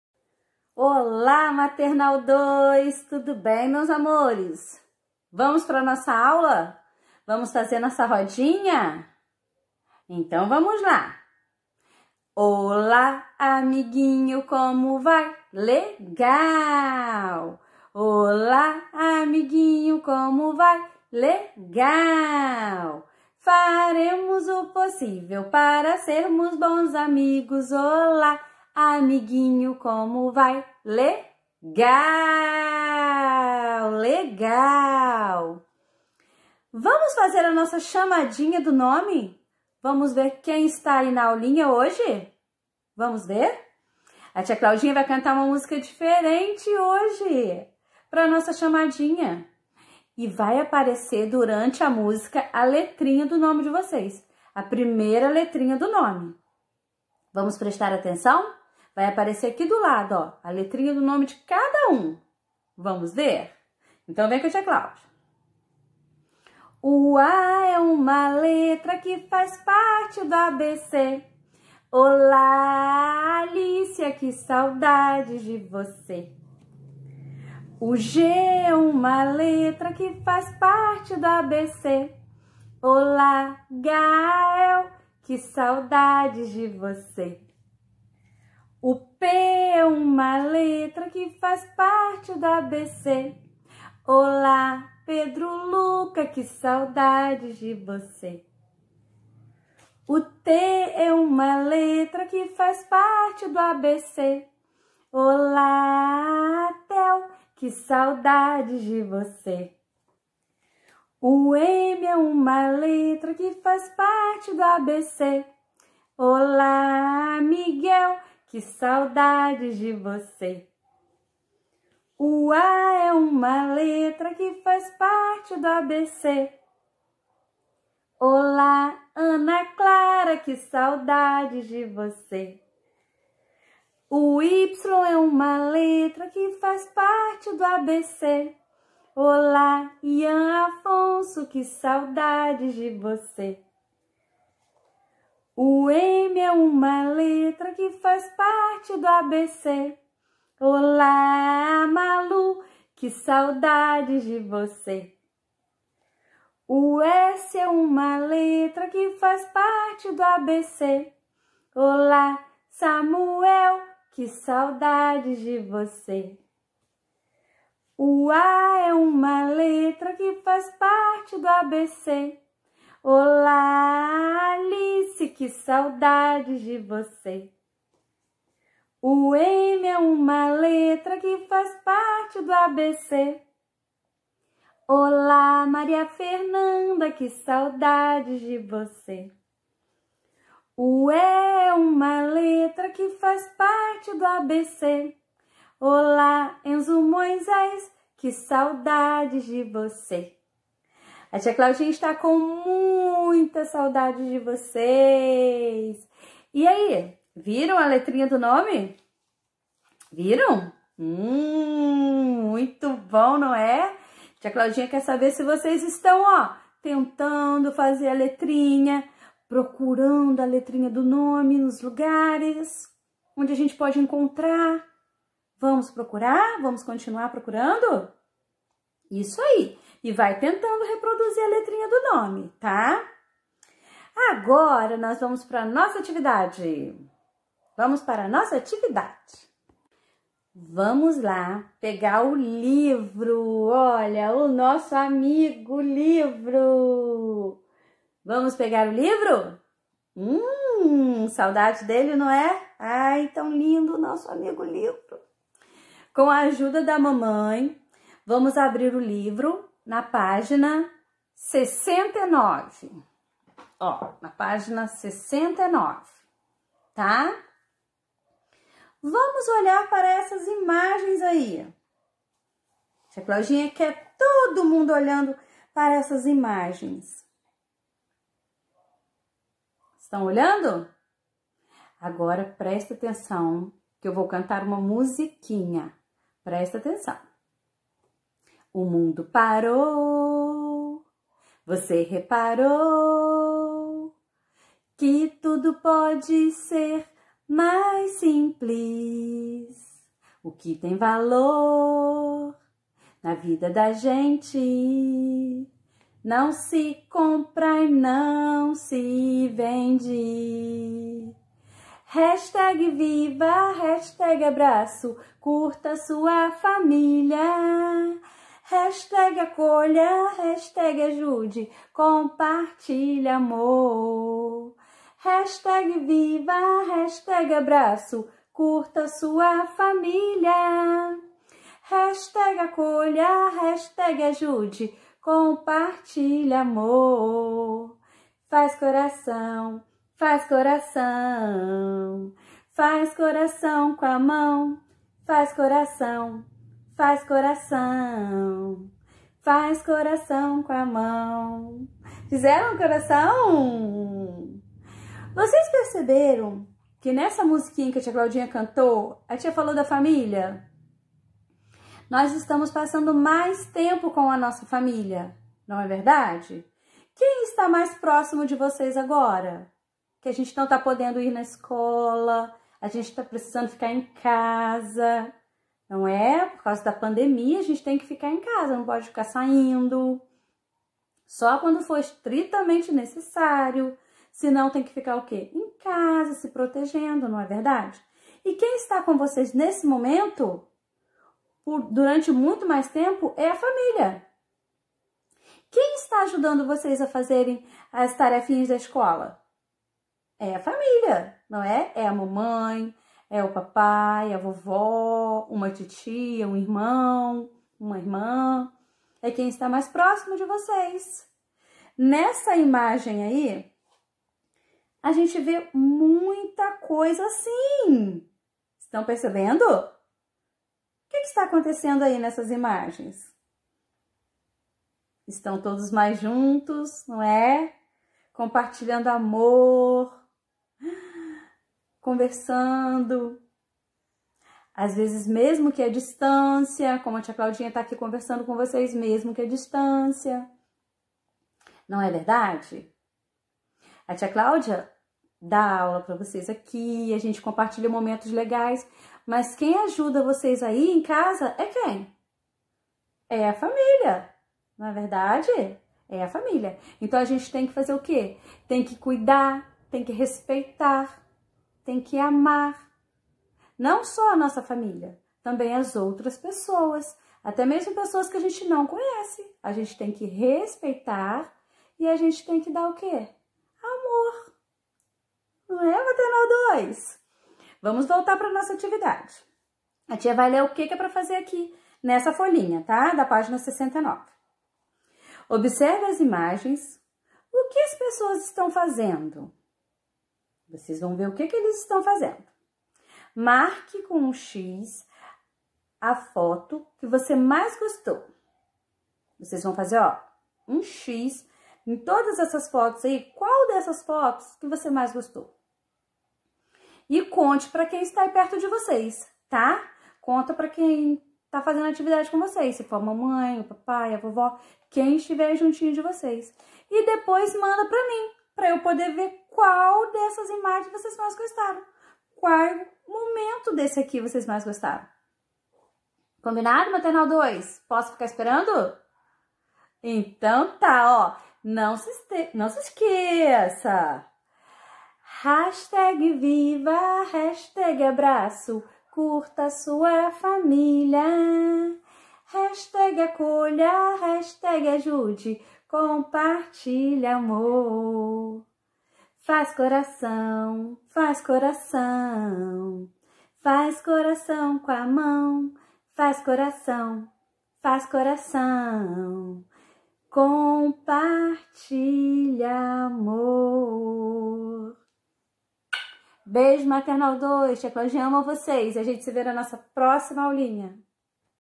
Maternal II - Aula 84 - Professora Cláudinha - Aula 04/11
Olá meus amores do maternal  ll!😍
Nossa aulinha está bem legal.
Vamos ver e cantar  a primeira letrinha do nome, fazer leitura de imagem da página 69 do nosso livro e  conversar sobre o carinho e respeito que devemos ter com a nossa família,amigos e
demais pessoas.
Beijinhos carinhosos, tia Claudinha 🥰